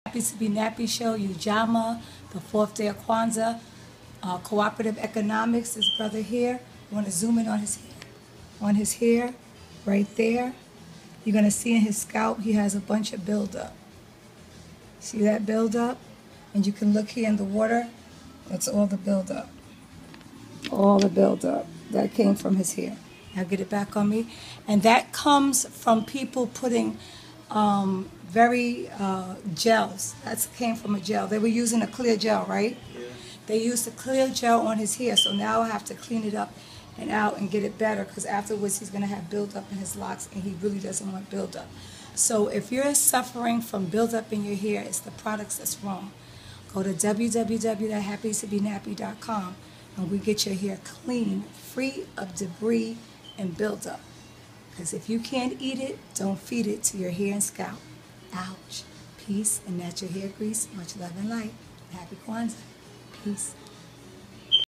Happy 2B Nappy Show: How 2 Get Rid Of Build Up :)
Host Queen Goddess Explains & Demonstrates How 2 Remove That Nasty Build Up On Your Scalp When Given Her Client A Loc Bath For More Info Go 2 Thx 4 Watch'n & Pls Don't 4Get 2 Subscribe Remember "If U Can't Eat Don't Feed it 2 Ya Hair & Scalp Ouch" Peace & Natural Hair Grease Much Luv & Light Alright :)